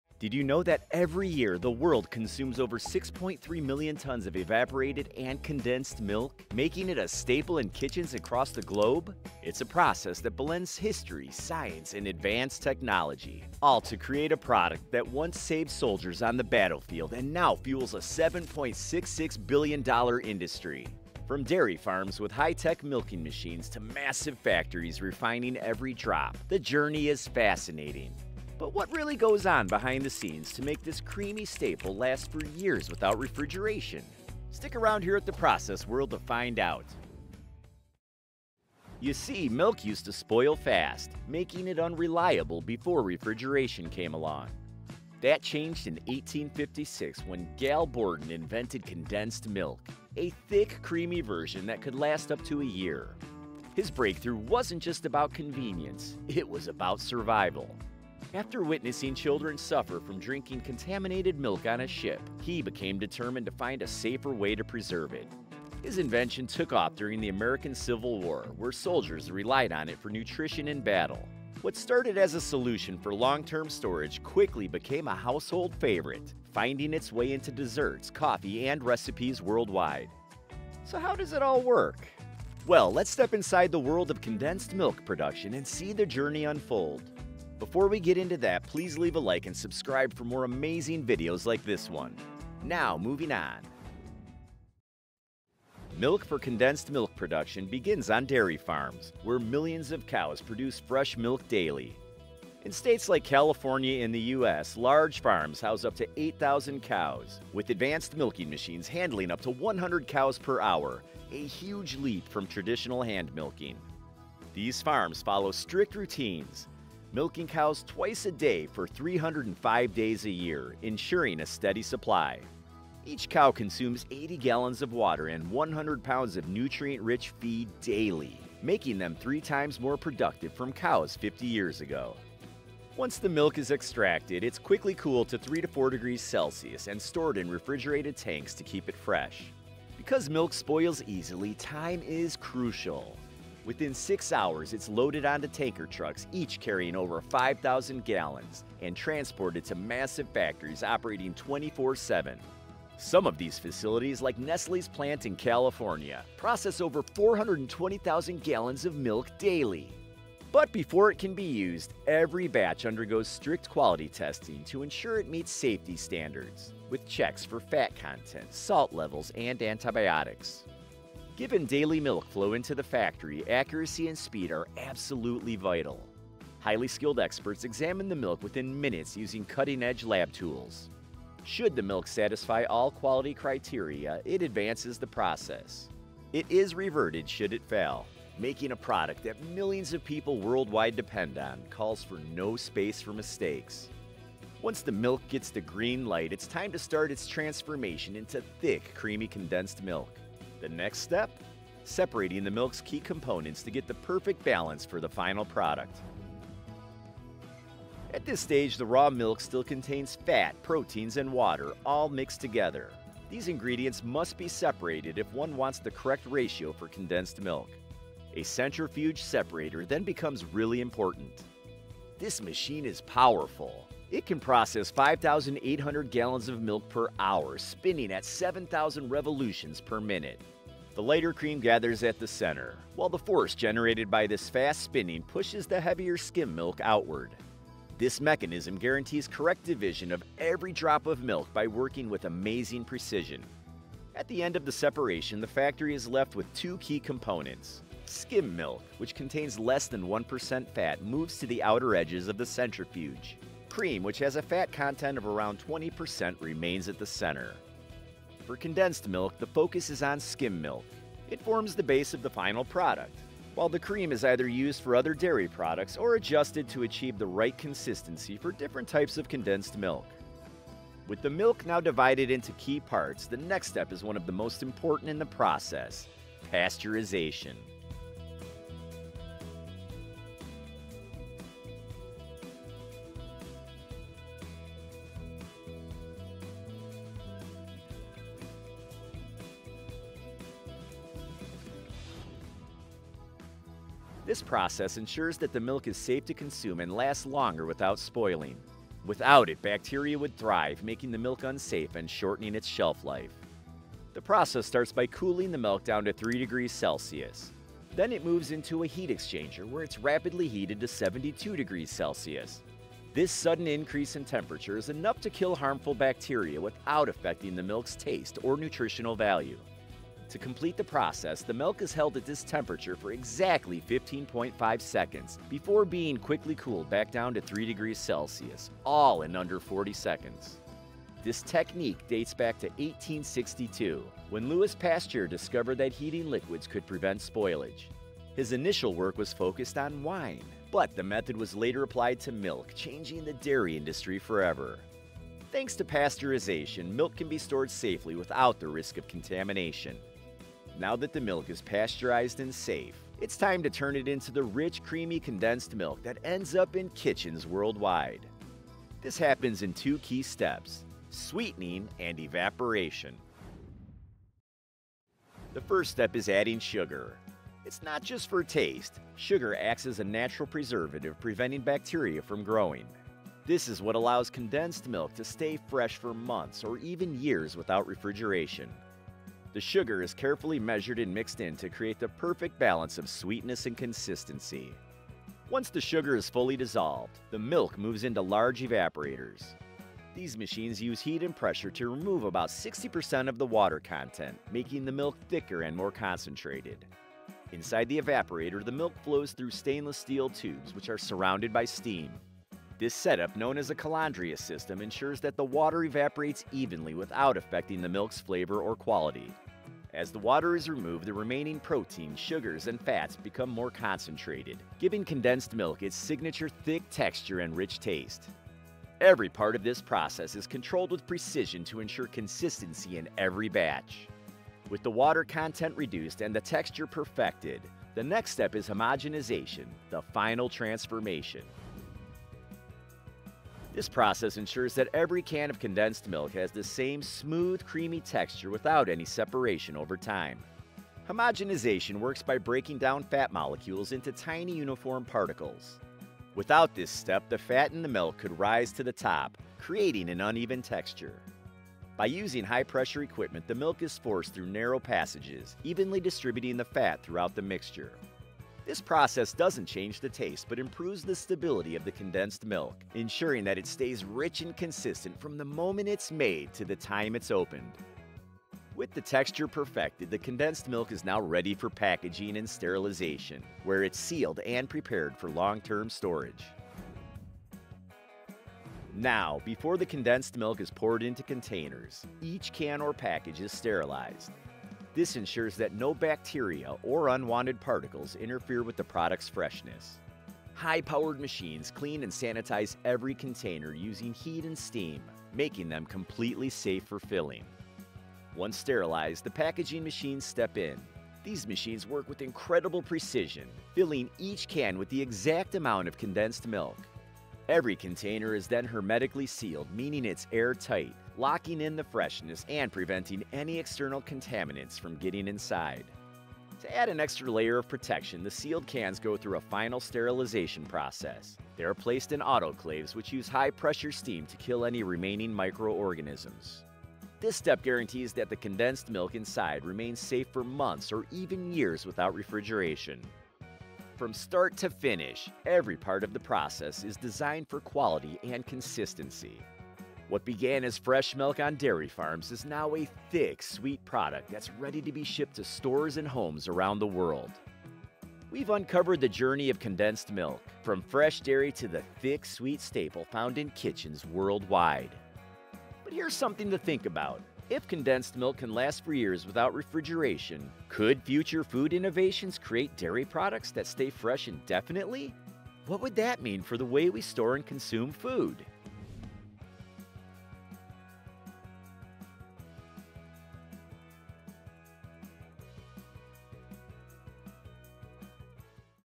How CONDENSED MILk is Made in Factory 🐄
Did you know that every year, the world consumes over 6.3 million tons of evaporated and condensed milk, making it a staple in kitchens across the globe. From dairy farms with high-tech milking machines to massive factories refining every drop, the journey is fascinating. But what really goes on behind the scenes to make this creamy staple last for years without refrigeration?and How Condensed Milk is Made in Factory? Stick around here at the process world to find out!

🔑 Key Moments:
00:00 INTRO
00:38 Historical Background
01:35 Production Process / From Dairy Farms to the Factory
03:27 Centrifugal Separation
04:52 Pasteurization
06 :04 Sweetening & Evaporation
07:12 Homogenization
07:57 Packaging & Sterilization
09:08 CONCLUSION